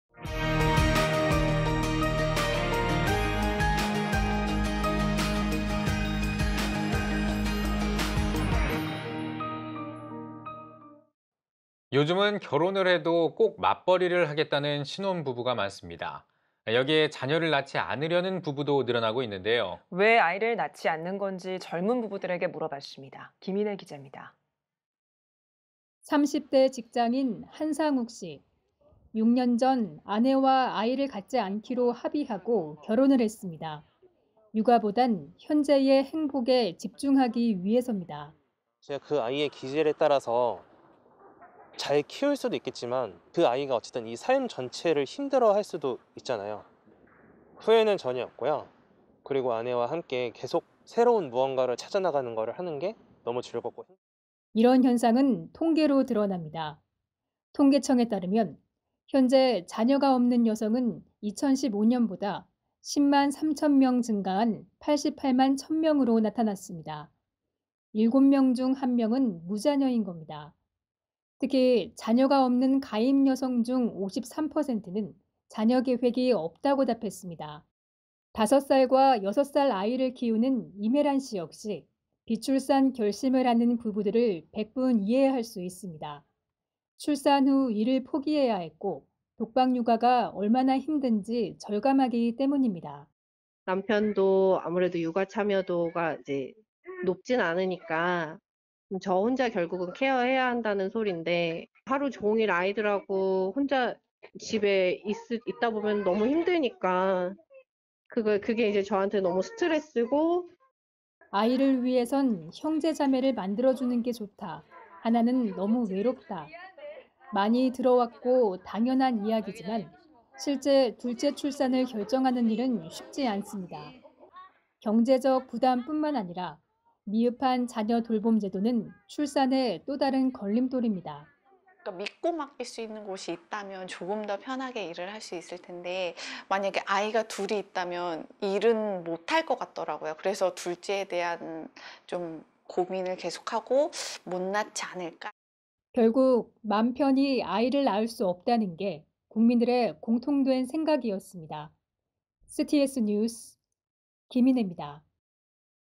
CTS연중기획 [다음세대가 희망입니다]⑤ - “아이 낳지 않겠다” 왜?(김인애) l CTS뉴스
앵커: 요새 같은 경제 상황에 결혼을 해도 꼭 맞벌이를 하겠다는 신혼부부가 압도적으로 많죠. 여기에 자녀를 낳지 않으려는 부부도 늘어나고 있습니다.

앵커: 왜 아이를 안 낳는 걸까요? 젊은 부부들에게 물어봤습니다. 김인애 기잡니다.

30대 직장인 한상욱 씨.
6년 전 아내와 아이를 갖지 않기로 합의하고 결혼을 했습니다. “육아 보단 현재의 행복에 집중하기” 위해섭니다.

INT 결혼 6년차 딩크족
아이의 기질에 따라서 잘 키울 수도 있겠지만 그 아이가 삶 전체를 힘들어 할 수도 있잖아요 후회는 전혀 없고요 아내와 함께 새로운 무언가를 찾아나가는 것을 하는 게 너무 즐겁고

이런 현상은 통계로 드러납니다.
통계청에 따르면 현재 자녀가 없는 여성은 2015년 보다 10만 3천명 증가한 88만 1천명으로 나타났습니다. 7명중 1명은 무자녀인 겁니다. 특히, 자녀가 없는 가임여성 중 53%는 자녀 계획이 없다고 답했습니다.

5살과 6살 아이를 키우는 임혜란 씨 역시 비출산 결심을 하는 부부들을 백분 이해할 수 있습니다. 출산 후 일을 포기해야 했고, 독박 육아가 얼마나 힘든지 절감하기 때문입니다.

INT 임혜란 / 두 자녀 엄마
남편도 아무래도 육아 참여도가 높지는 않으니까 저 혼자 결국은 케어 해야 한다는 소리인데 하루 종일 아이들하고 혼자 집에 있다 보면 너무 힘드니까 그것이 저한테 너무 스트레스고

‘아이를 위해선 형제자매를 만들어주는 게 좋다, 하나는 너무 외롭다’
많이 들어왔고, 당연한 이야기지만 실제 둘째 출산을 결정하는 일은 쉽지 않습니다. 경제적 부담뿐만 아니라 미흡한 자녀 돌봄제도는 출산의 또다른 걸림돌입니다.

INT 강수미 간사 / 외동딸 엄마, 서울YWCA 회원활동팀
믿고 맡길 수 있는 곳이 있다면 조금 더 편하게 일을 할 수 있을 텐데 만약에 아이가 둘이 있다면 일은 못할 것 같더라고요 그래서 둘째에 대한 고민을 계속하고 못 낳지 않을까

결국 맘 편히 아이를 낳을 수 없다는 게 국민들의 공통된 생각이었습니다.
CTS뉴스 김인애입니다.